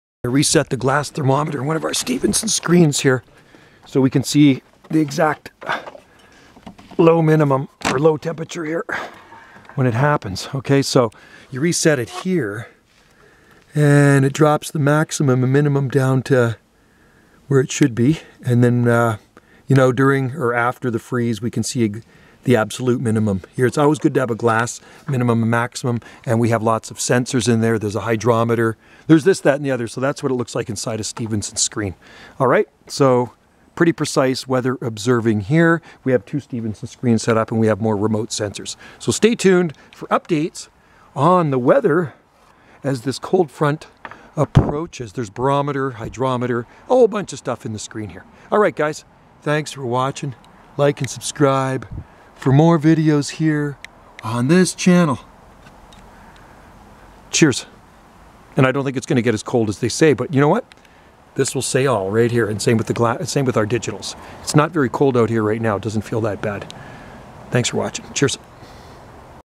Resetting min/max glass thermometer in a Stevenson Screen weather box
Accuracy is most important and this is the calm before the deep freeze. Seasonal here today but that is about to change